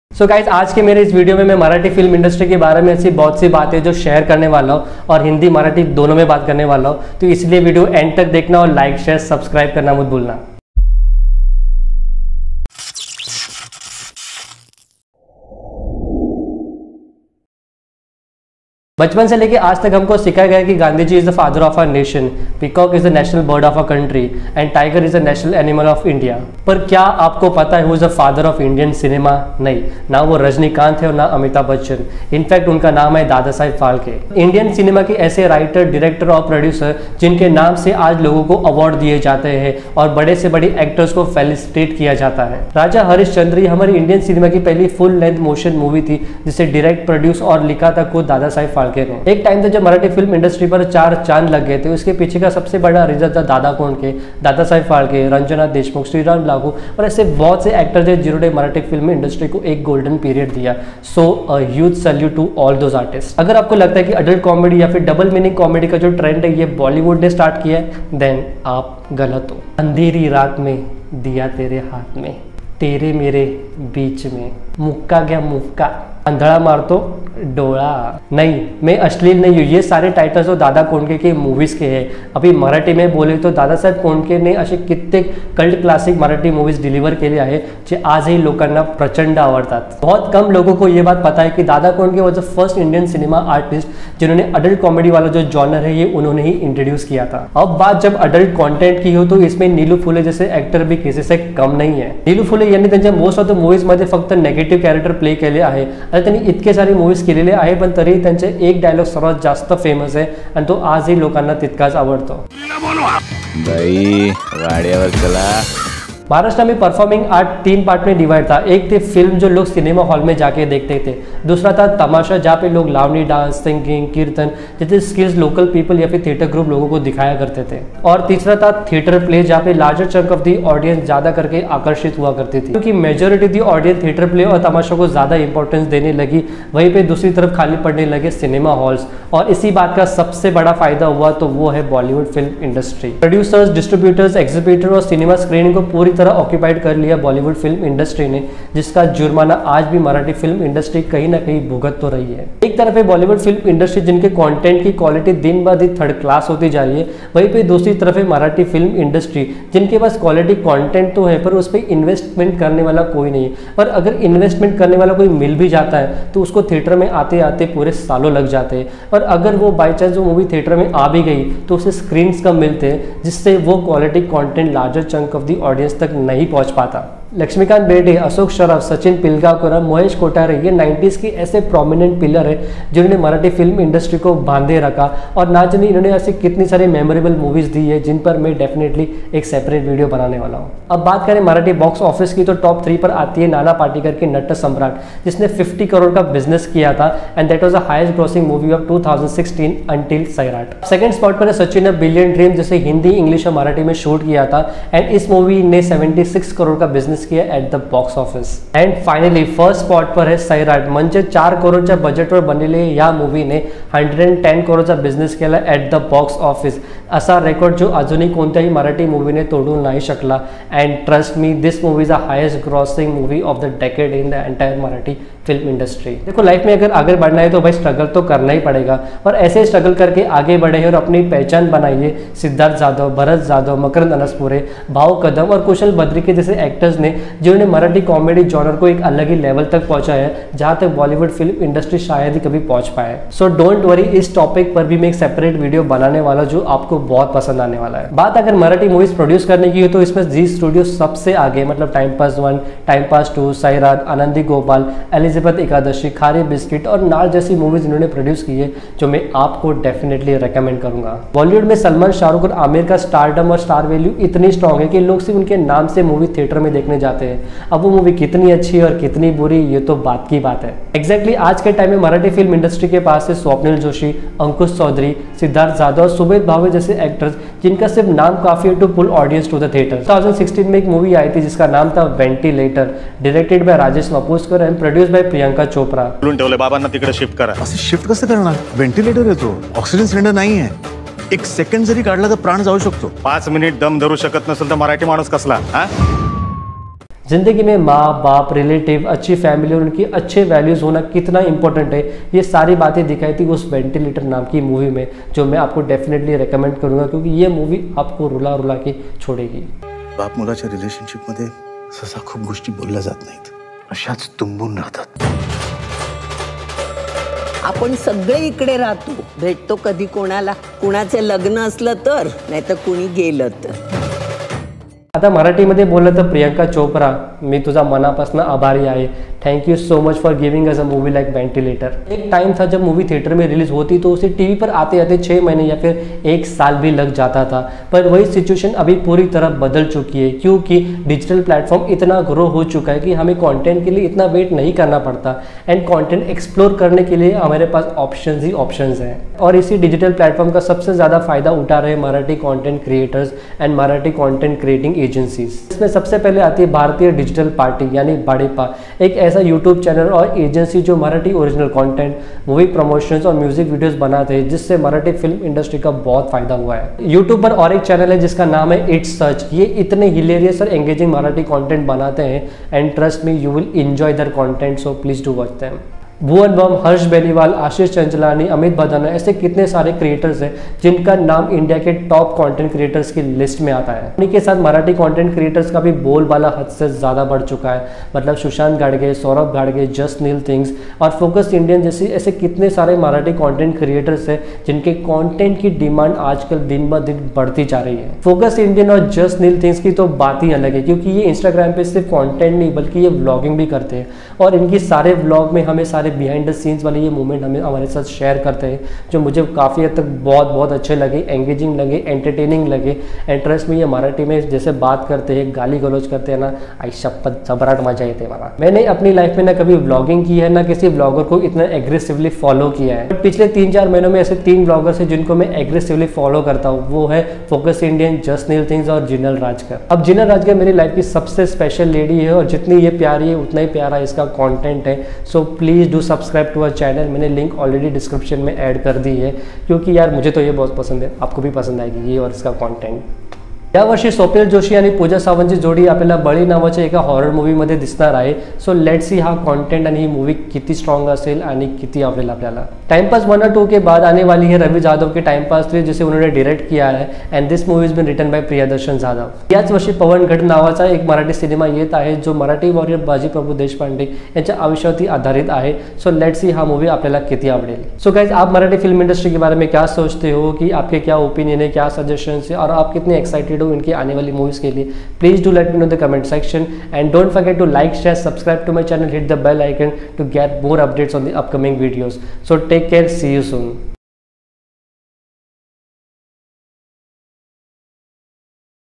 Support Marathi Film Industry | Support Marathi Content Creators | Focused Indian #32
Hi Guys,

Here i am presenting my research and opinion on History of Marathi film industry, Rise and Fall of Marathi film industry, Marathi content creators and their contribution to the entertainment industry.

Video Credit - Ashish Dangle
YT Consultant - Social Bugz